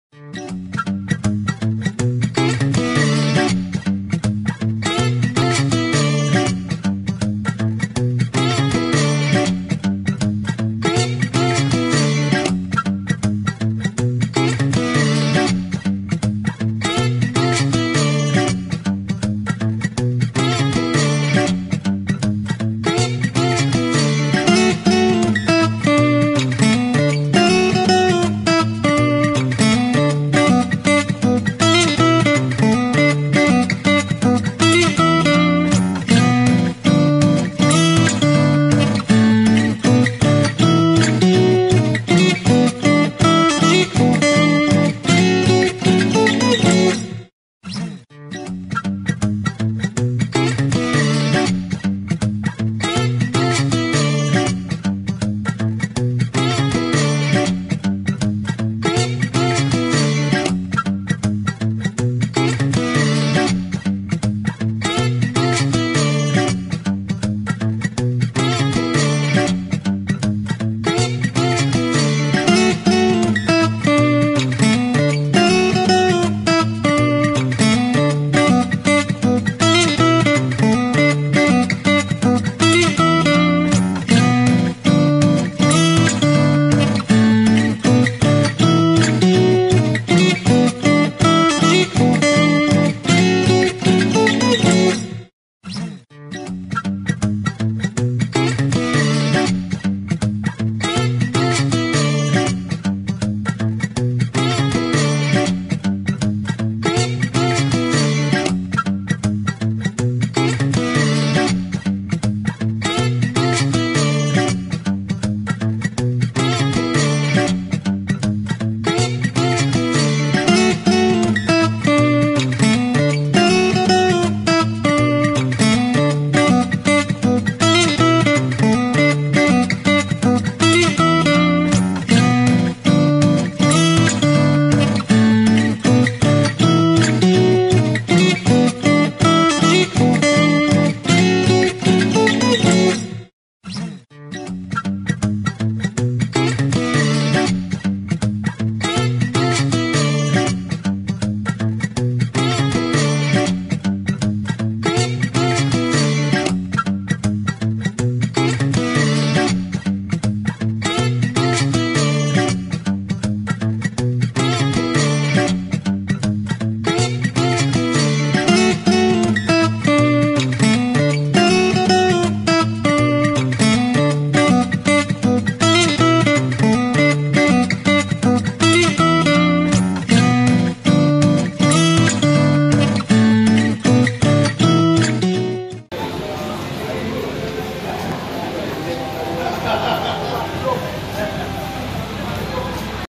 AYAM GEPUK PAK GEMBUS LAKE FIELD SUNGAI BESI
Kalau korang suka pedas cili api... Datang la sini... Memang superb... Datang sebelum or selepas lunch.. Ramai gila kat sini.. Tapi berbaloi.. Selamat mencuba guyss..